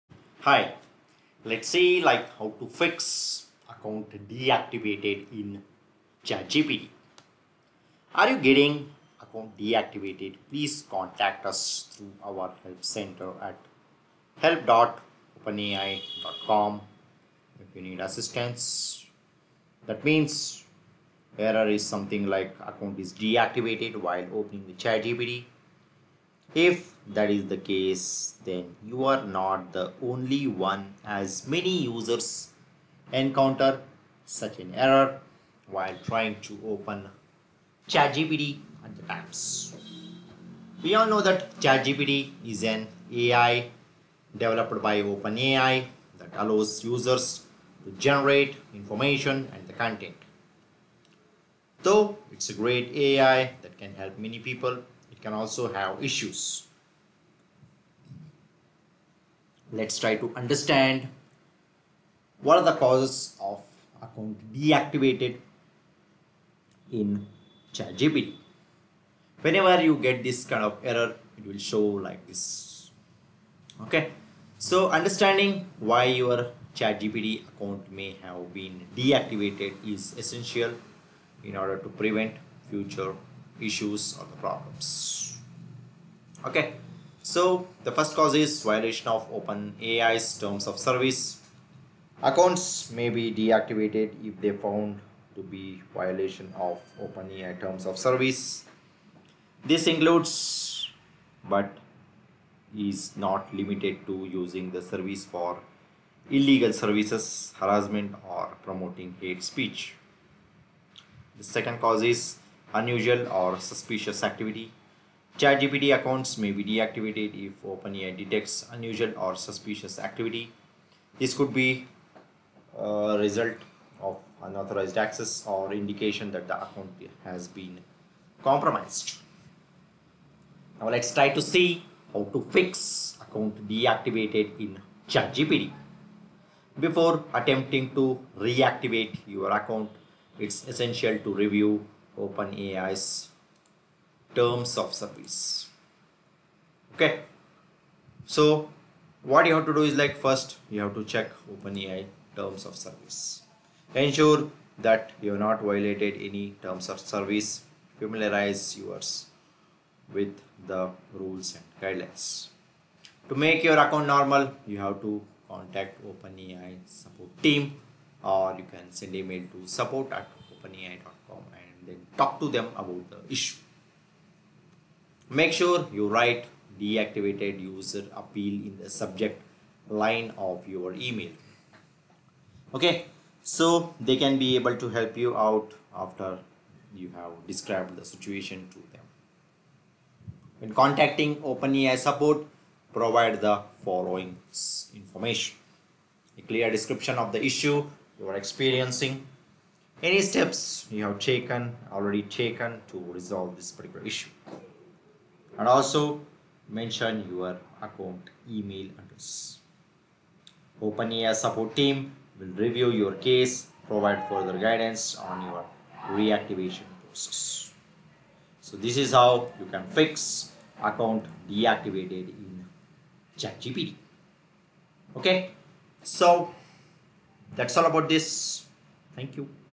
How To Fix Account Deactivated In ChatGPT
Are you getting “Account deactivated, Please contact us through our help center at help.openai.com if you need assistance, (error=account_deactivated)” while opening ChatGPT?If that is the case then you are not the only one as many users encounter such an error while trying to open ChatGPT at times.ChatGPT is an AI developed by Openai that allows users to generate information and content.Though it is a great AI that can help many people, it can also have issues.